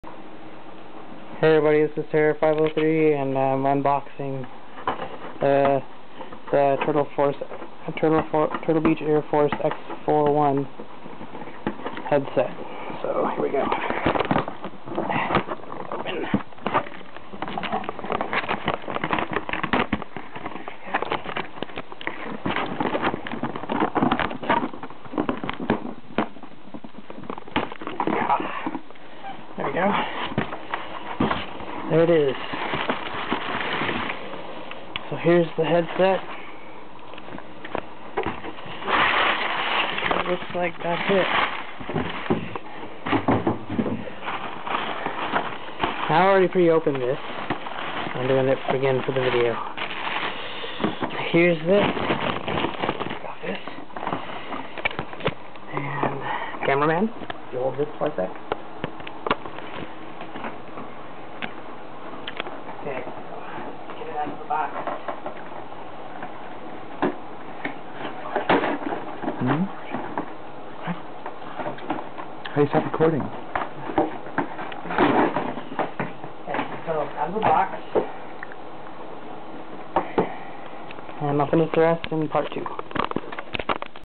Turtle Beach earforce x41 Unboxing part 1
New head set i bought around 200 dollars with shipping,takes about 7 buisness days for it to get to the destination,really awesome headset bass boost,game sounds in the headset(never have to turn your tv down agian),xboxlive chat also in the headset,